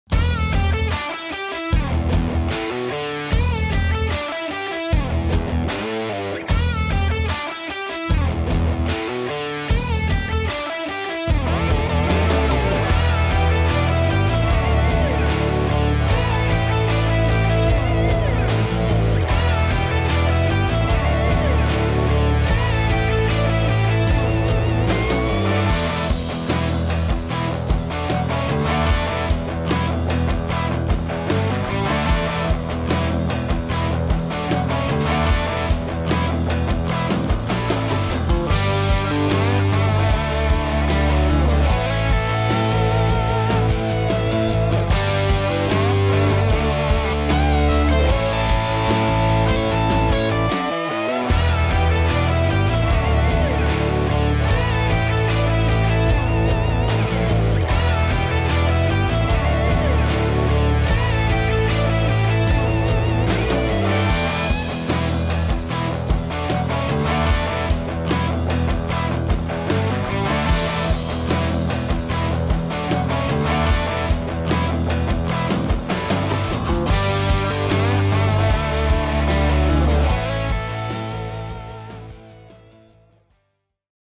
Protezioni faretti alluminio plexiglas 4 colori moto
Protezione faretti per donare un look esclusivo ed originale alla propria moto migliorando allo stesso tempo la sicurezza per i propri faretti avendo una protezione aggiuntiva

Sistema di apertura rapida per consentire l'alzamento della protezione superiore